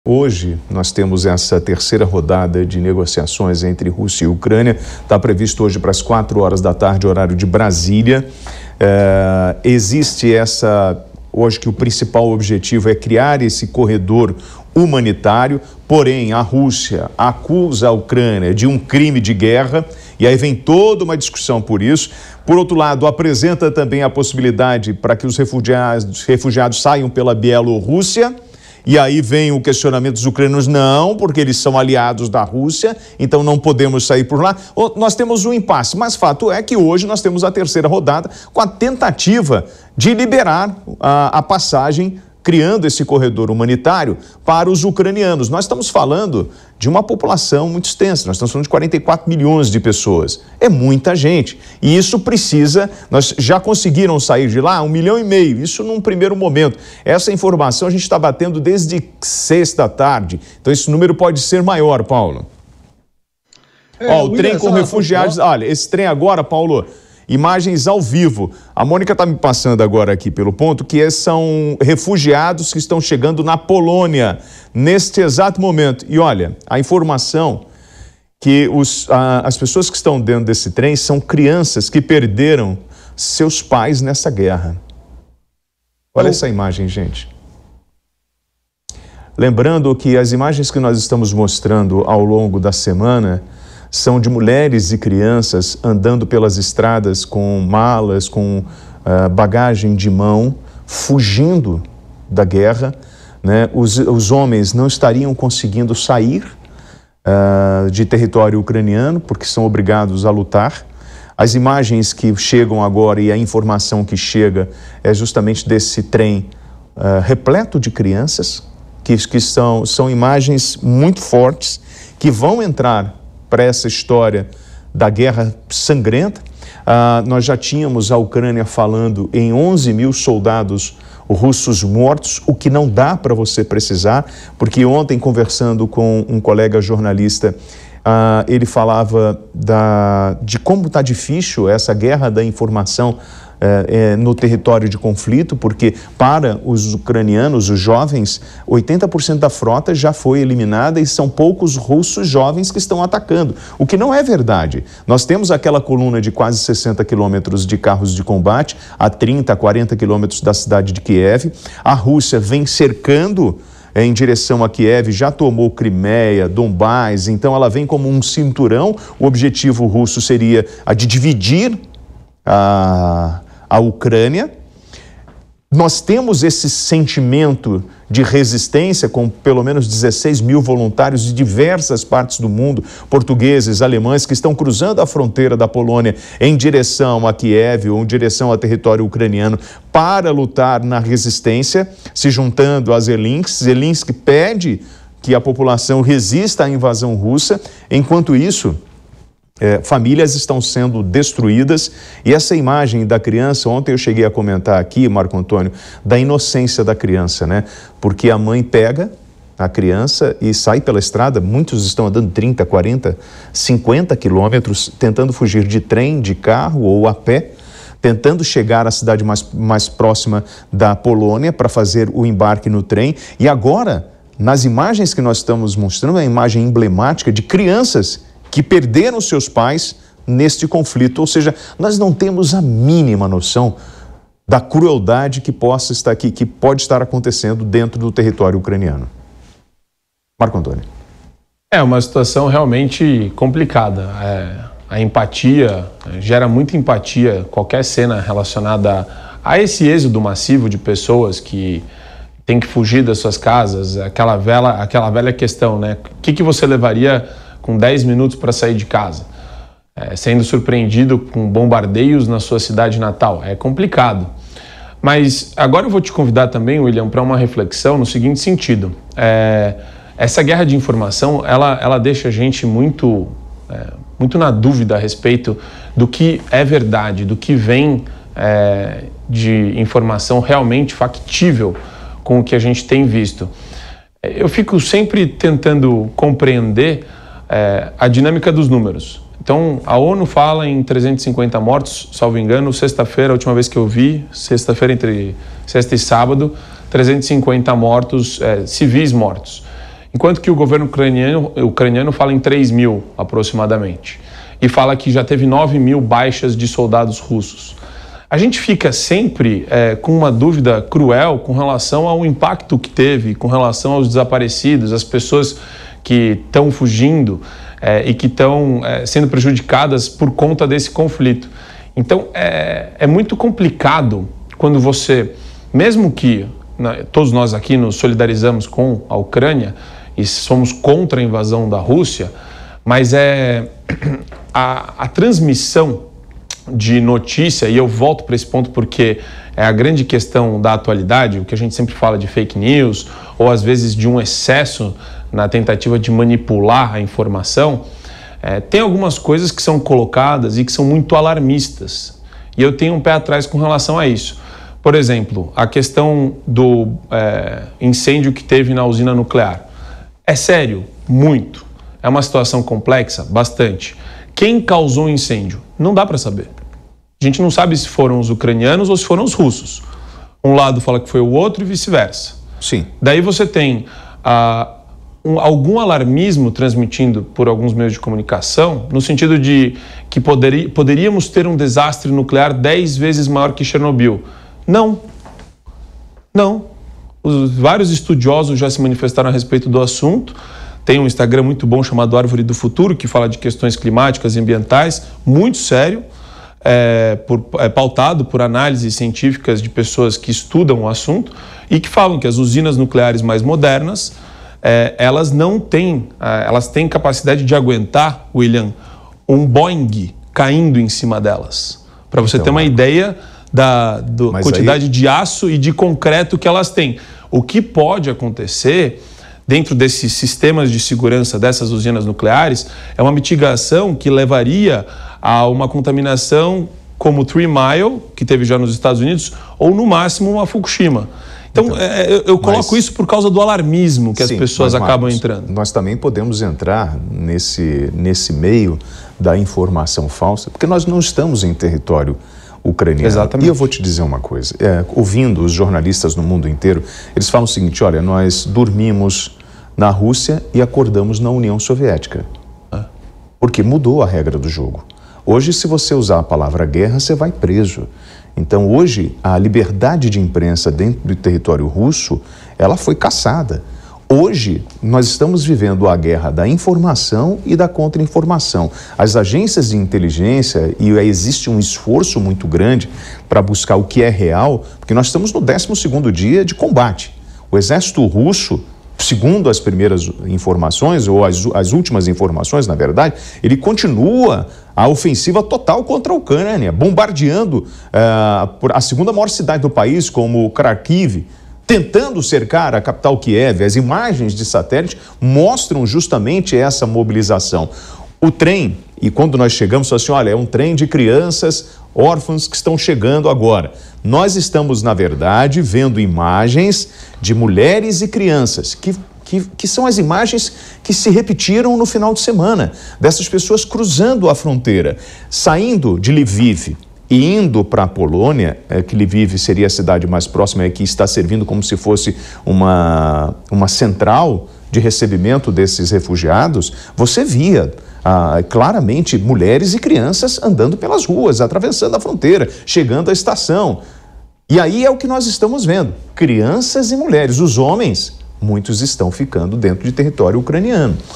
Trem chega com crianças na Polônia
Um trem com crianças ucranianas órfãs e mulheres chegou na Polônia nesta segunda-feira, 7. Przemysl, na Polônia, cerca de 10 km da Ucrânia, é um dos pontos principais de receberem os mais de 100 mil pessoas que fogem diariamente da Ucrânia, invadida pela Rússia há mais de uma semana.